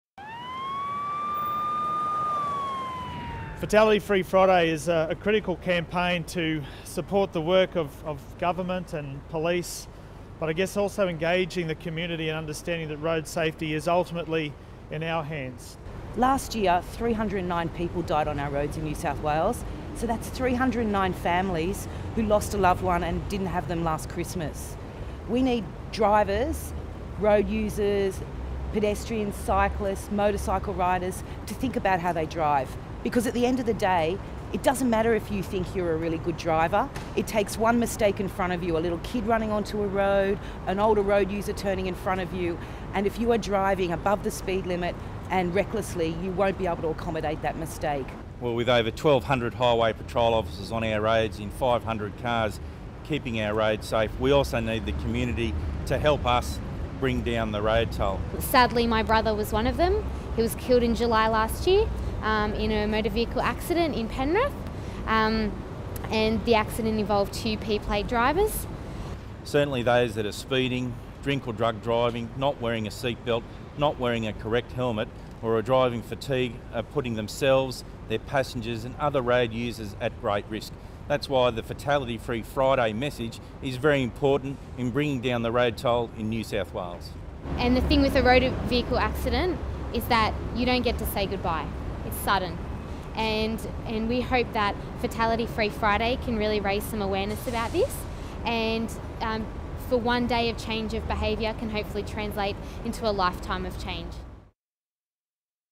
Fatality Free Friday launch
This Friday (29 May 2015) is Fatality Free Friday. Fatality Free Friday is an initiative of the Australian Road Safety Foundation which calls for road users to make a promise to themselves, their family and friends to consciously drive safely and obey road rules.

All emergency services, including police, ambulance and fire and rescue, will unite alongside safety advocates imploring road users to take the pledge.

Last year, 1155 drivers, pedestrians, cyclists and motorcycle riders began a journey ending in tragedy. The number of people directly and indirectly affected is incredible.

As police, sometimes our job involves issuing a warning or a fine, and sometimes we have to charge motorists in the hope we change their behaviour to minimise the risk of serious injury or death.

We will continue to focus on reducing road trauma by awareness of the fatal five: speeding, drink-or-drug driving, seatbelt use, driving distracted and driving tired.

Road users are able to make their road safety pledge by visiting